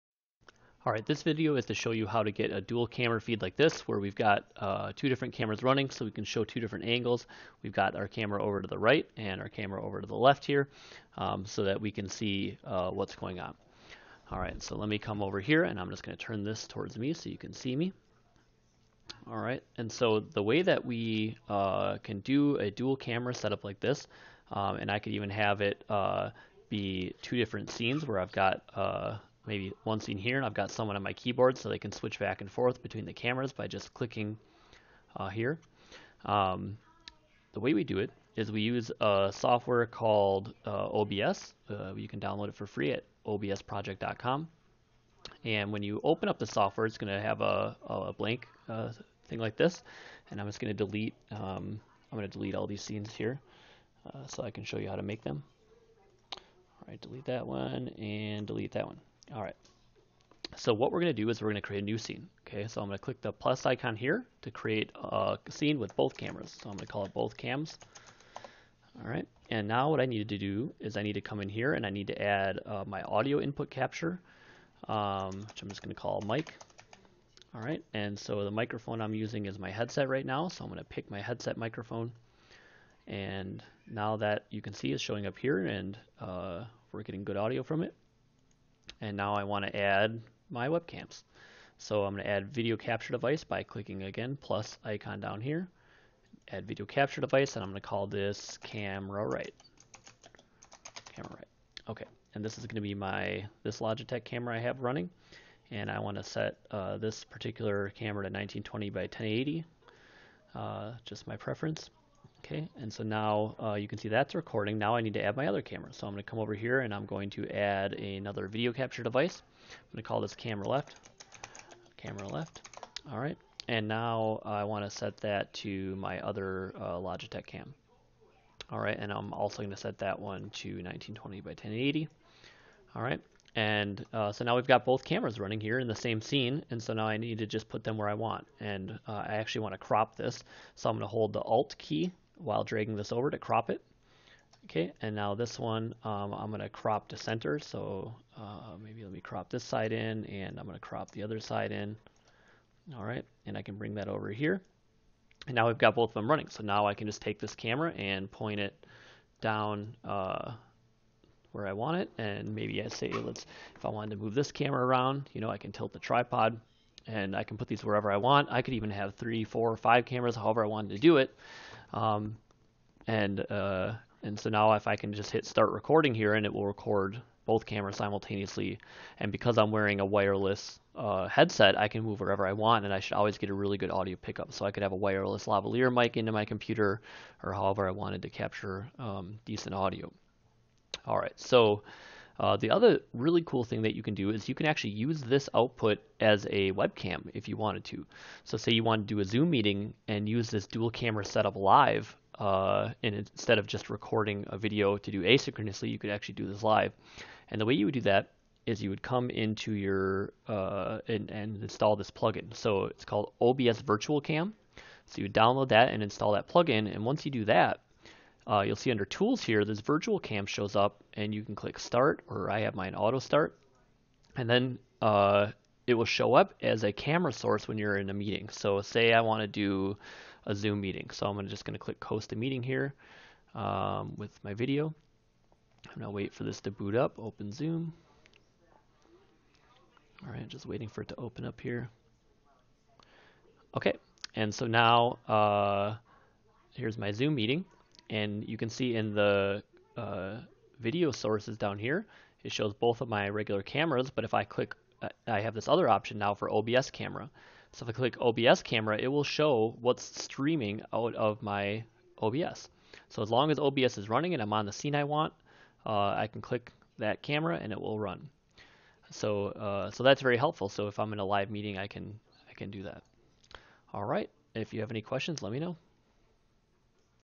How to record or stream with multiple cameras (OBS)
Using multiple webcams is easy with OBS, just put them in your scene and you're readying to go! If you want to use OBS as a webcam source you just have to install the virtual camera plugin.